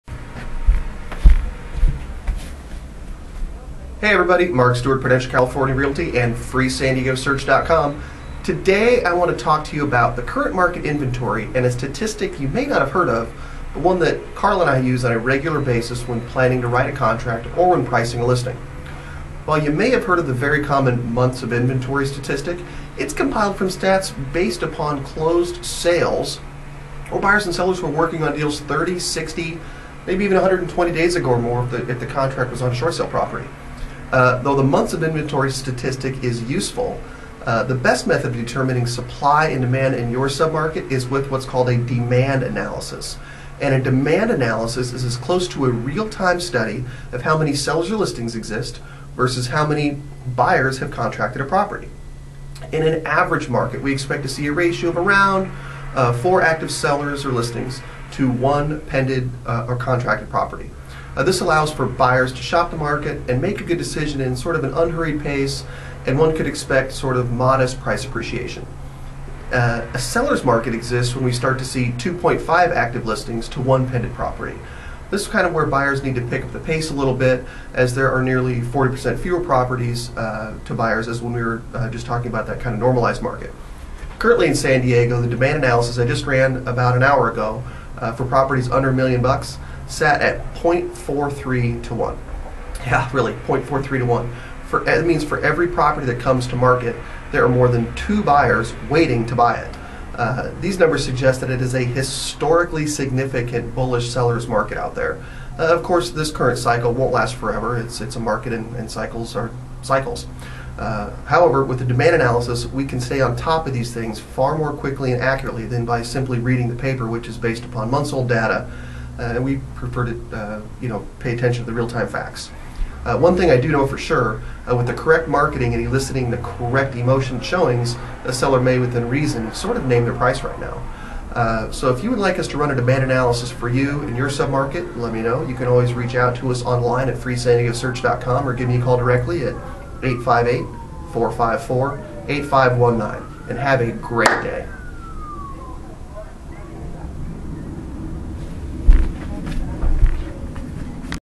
What is a Market Inventory Demand Analysis
Mark Stuart discusses what a real estate market inventory demand analysis is and how it differs from traditional market inventory statistics.  : for more information on San Diego real estate in your area.